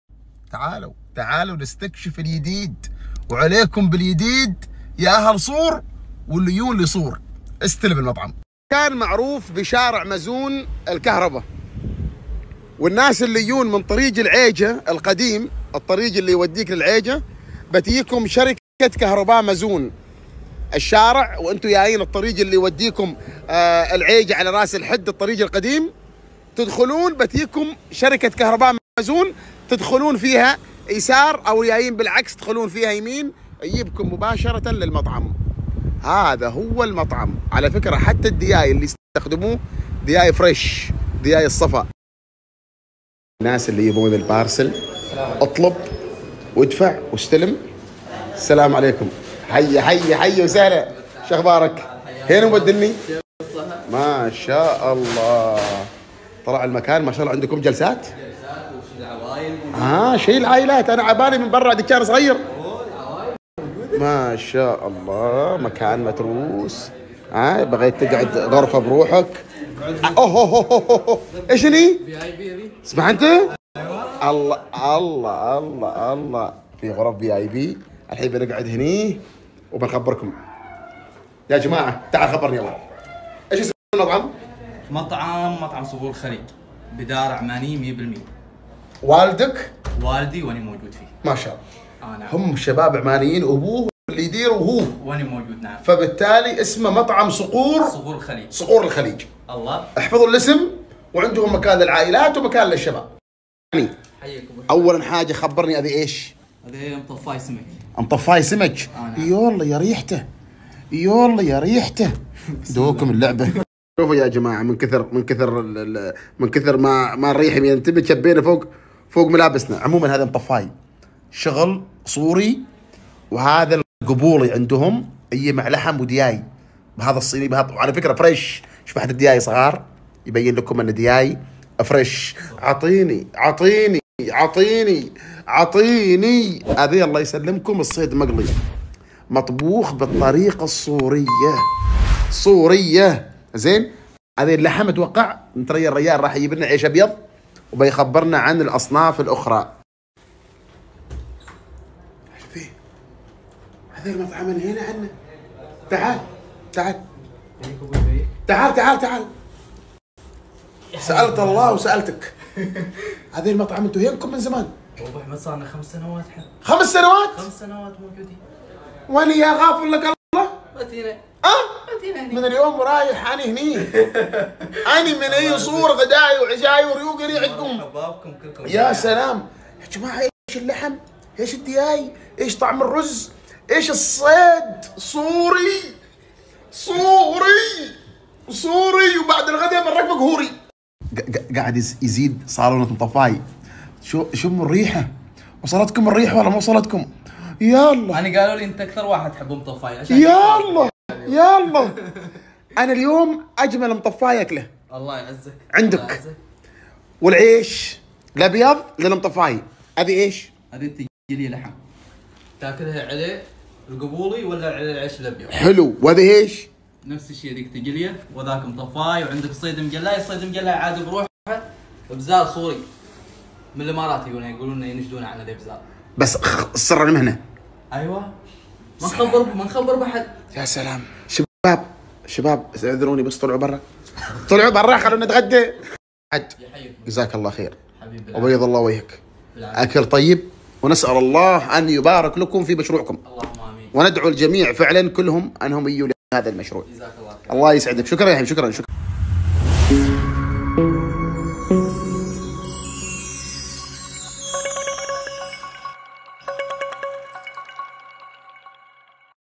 محمد_المخيني/مشروع عماني مطعم صقور الخليج للمأكولات العمانية و اليمنية و الوجبات السريعة في صور
📞 95755979